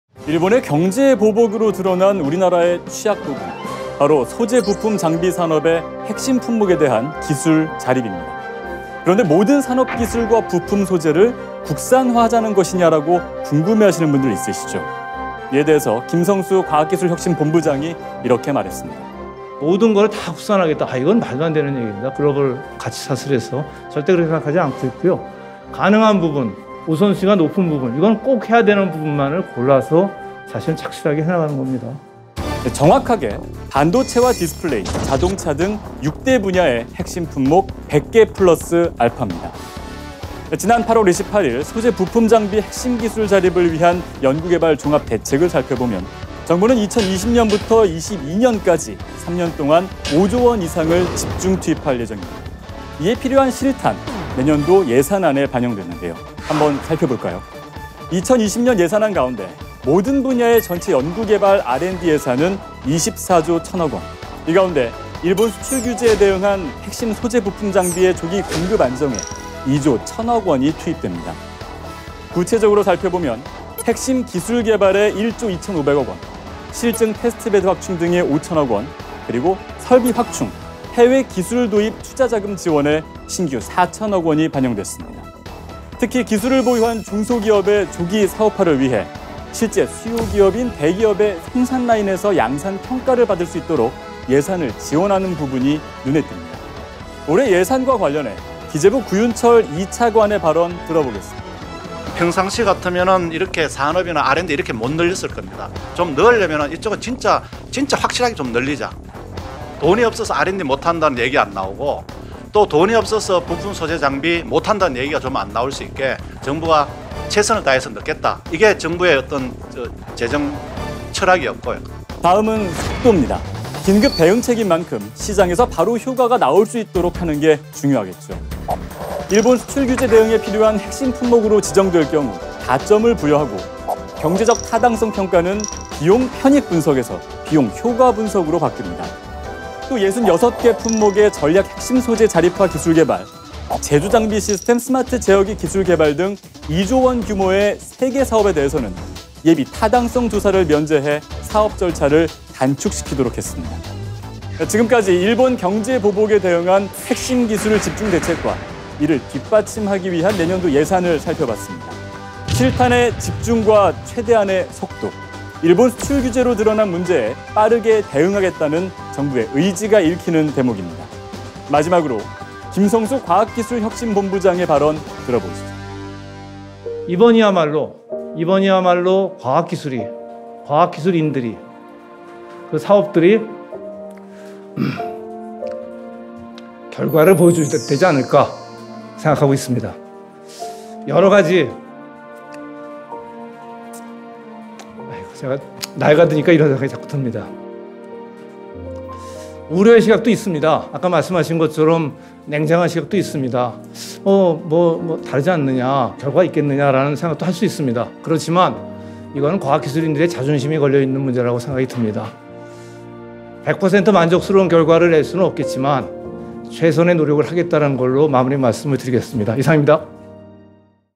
"핵심부품 국산화로 日 보복 돌파" 2020년 정부 예산안 완전 분석
일본 수출규제, 경제보복 조치 이후 우리 정부는
핵심 소재·부품·장비의 국산화를 추진하기 위한 대책들을 발표했습니다.
구체적인 ‘탈일본’ 추진 방향과 계획이 포함된 2020년 예산안
함께 살펴볼까요?